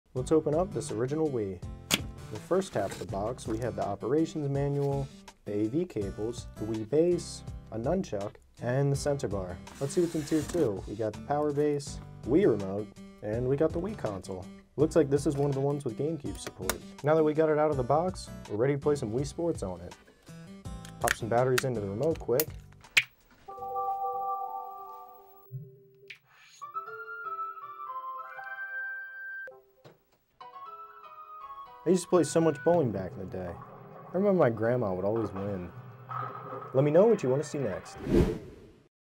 Unboxing a Wii Console From 2006!
Let’s go back to 2006 when the Wii first launched! The Wii was a revolutionary system that changed the way we played together! Whether you played alone or with others, the Wii had a lasting impact on gaming that is remembered to this day. What’s one of your favorite Wii memories, and why?